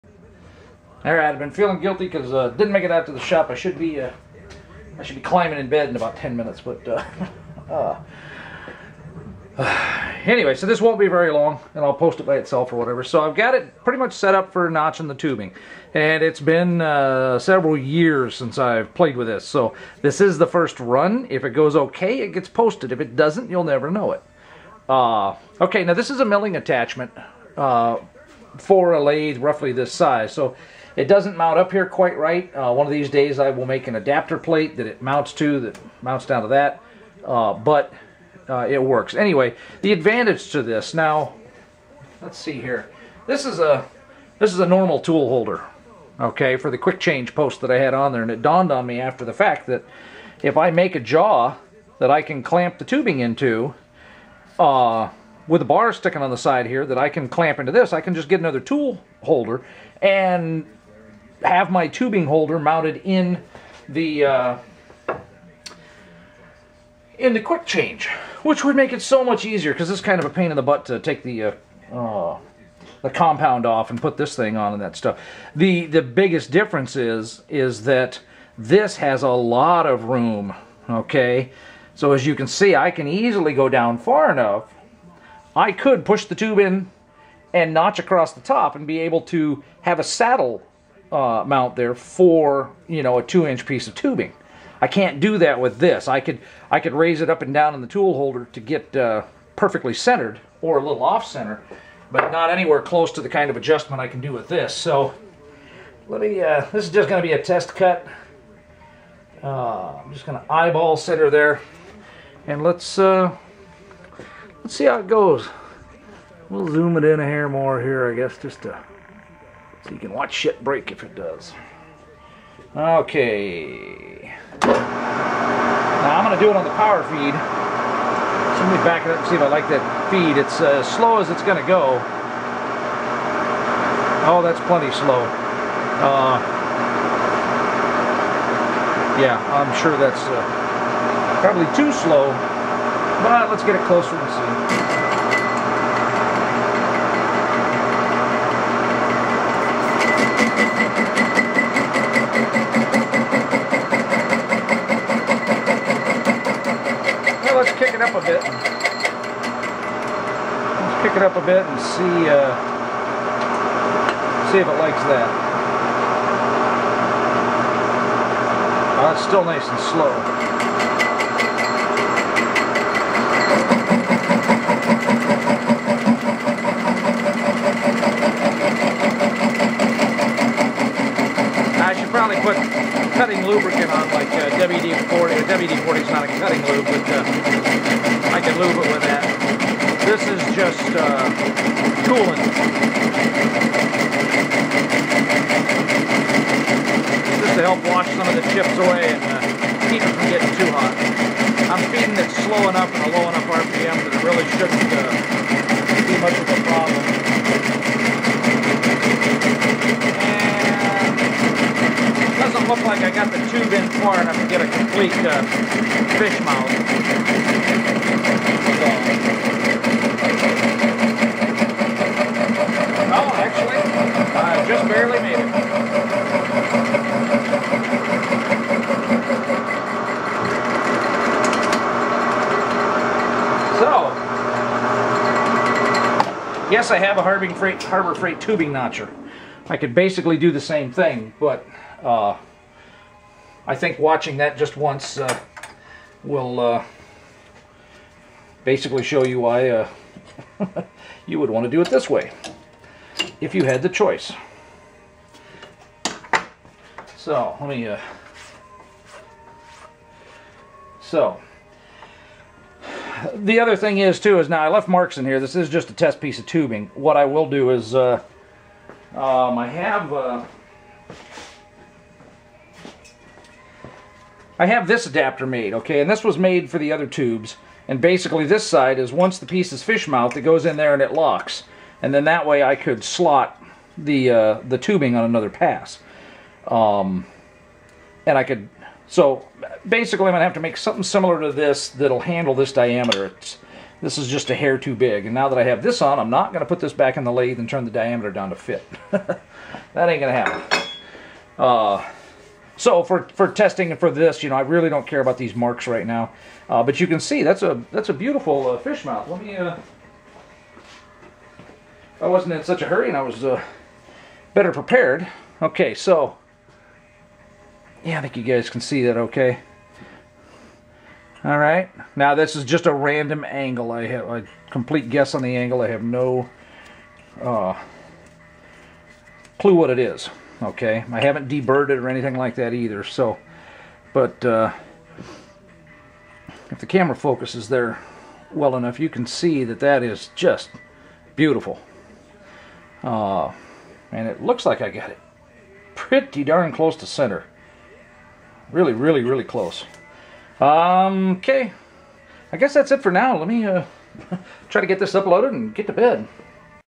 Tube Notching 2
Second segment for the tubing notching.  I mounted the milling attachment to my lathe and through a piece of black pipe in for a quick demo.  Setting the correct angle is going to take some black magic of some kind, but I'll worry about that when I know what angle to use.